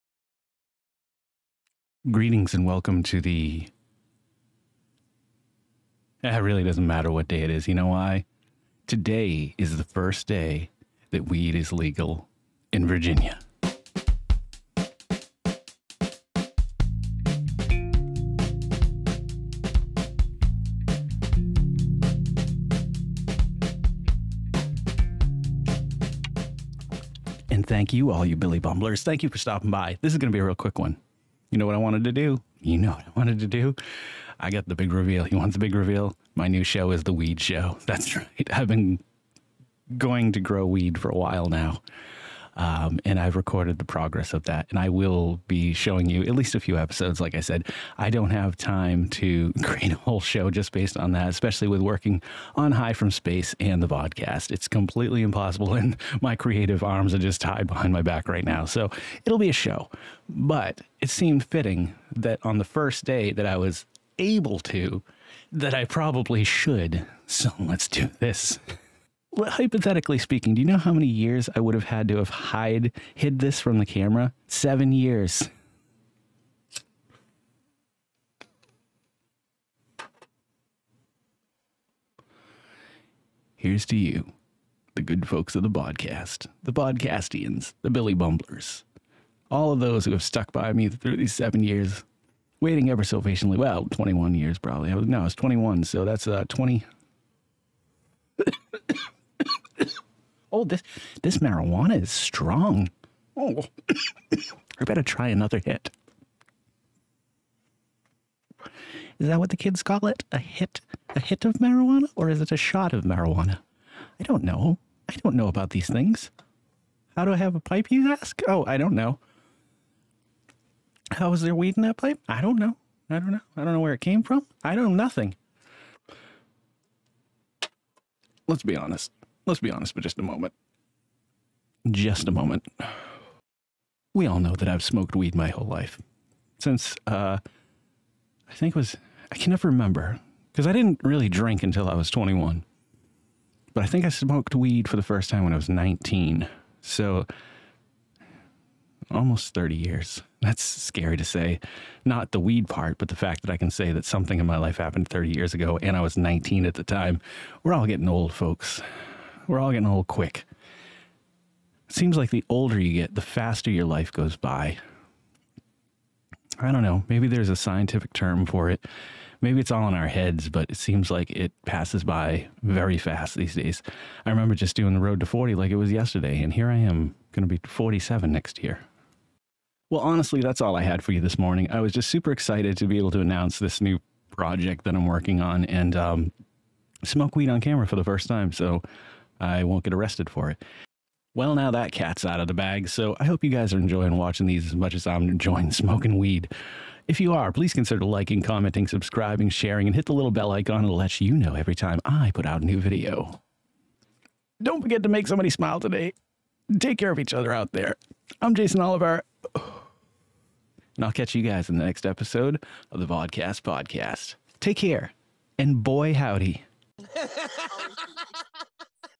The Vodkast Podcast - The Cat's Out of the Bag!
Today marks a significant moment in my life. Today, is the first day that I can consume cannabis, legally. It's about time Virginia. July 1st marks the first day of legal, adult marijuana possession and consumption. It will be a day, and a 4th, to remember. Thanks for stopping by!